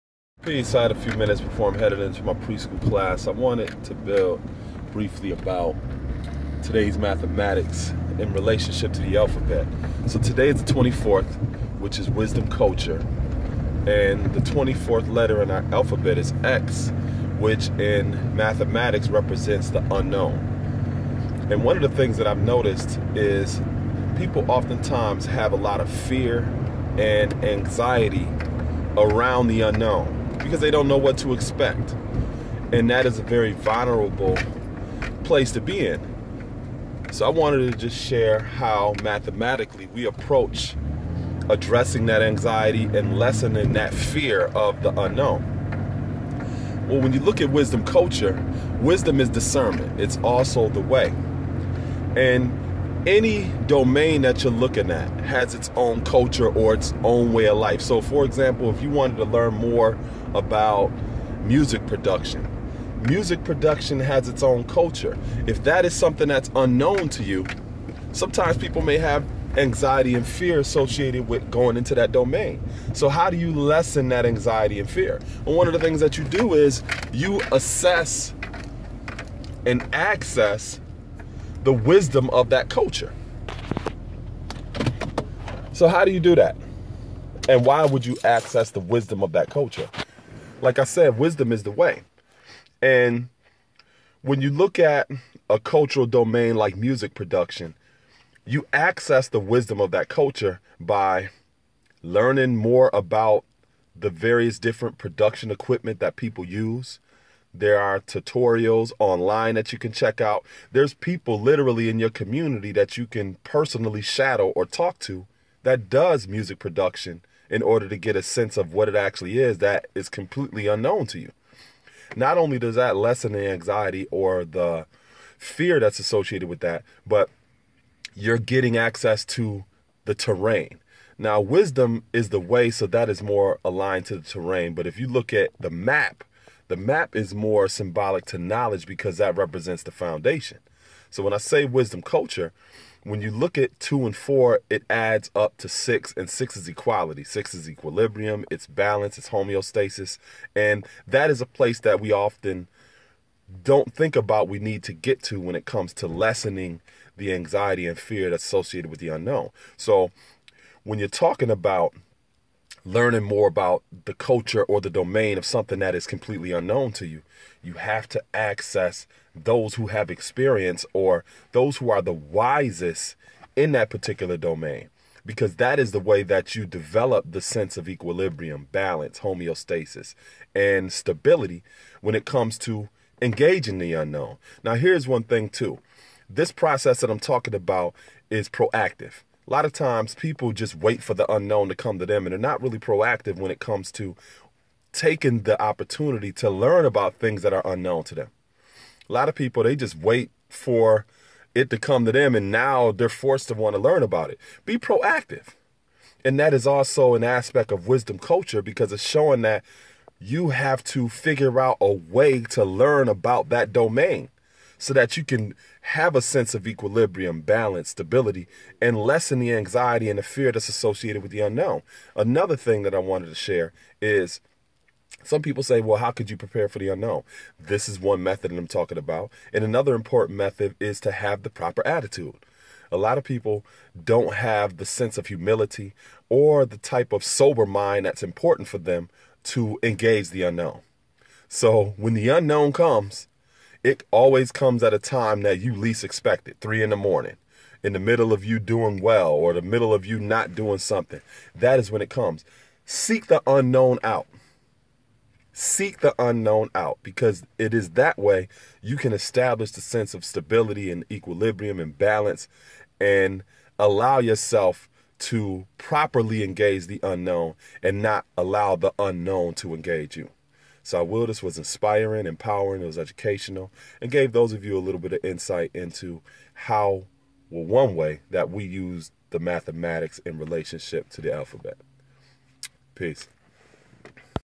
Wisdom Culture [24] & the Unknown (AtlantisBuild)
In this video I discuss the relationship between Wisdom Culture, Equality and the Unknown.